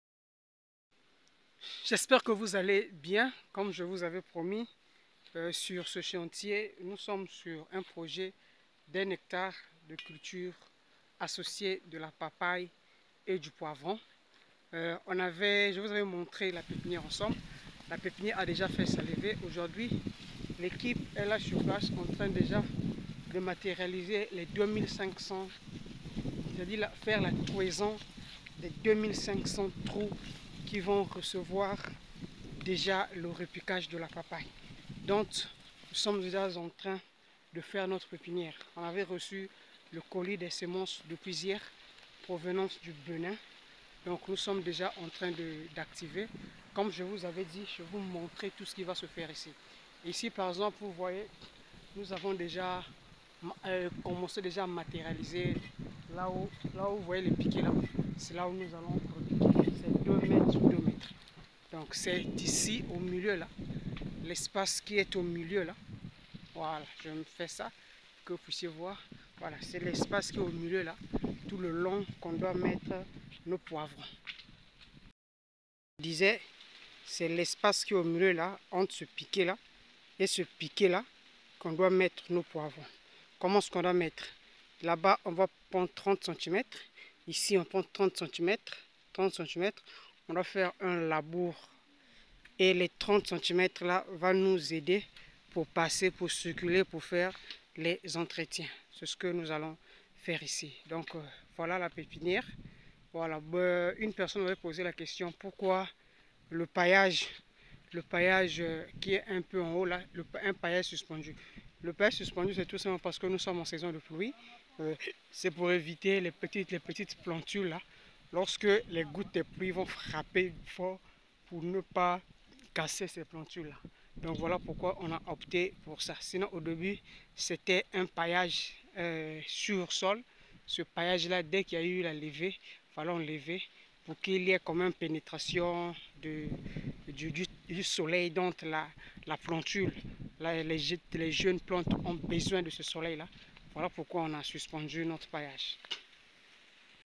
Comment gagner 15.000.000 sur un hectare
Dans cette vidéo je vous montre comment gagner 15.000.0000 sur un hectare en utilisant les cultures associé,